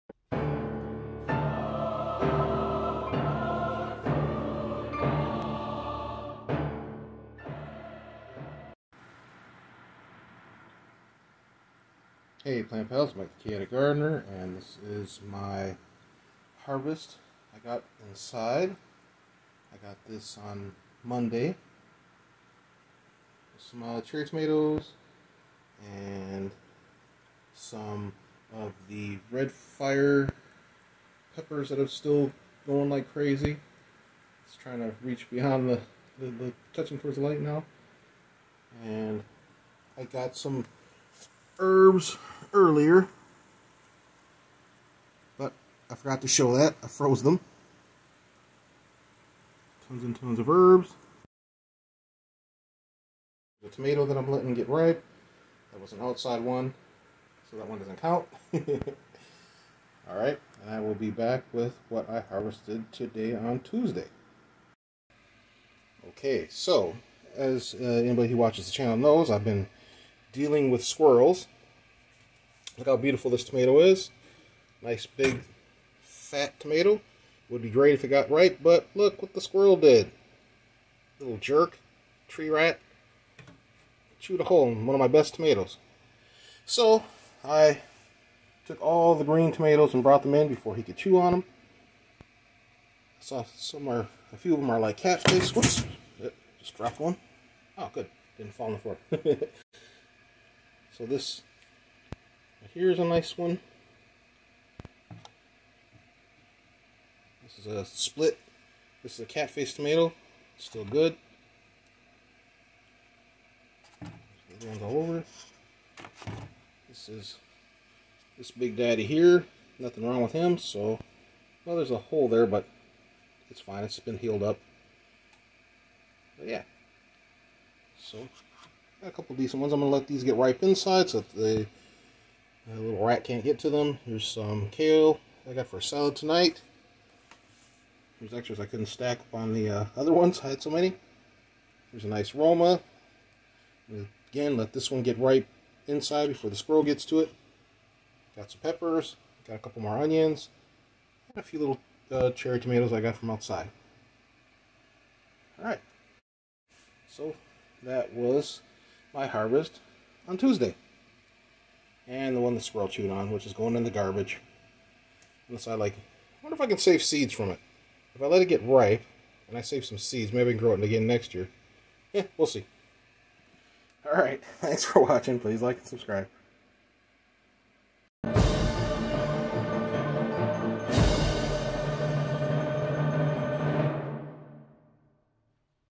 Saving Tomatoes from Squirrels
Had to harvest my tomatoes green, because the squirrels
steal them as soon as they ripen.